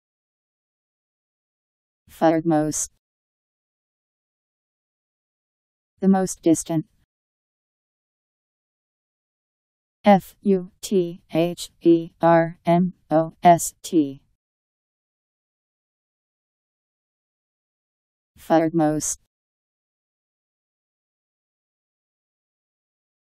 Futhermost Meaning
Video shows what futhermost means. The most distant..  Futhermost Meaning. How to pronounce, definition audio dictionary. How to say futhermost. Powered by MaryTTS, Wiktionary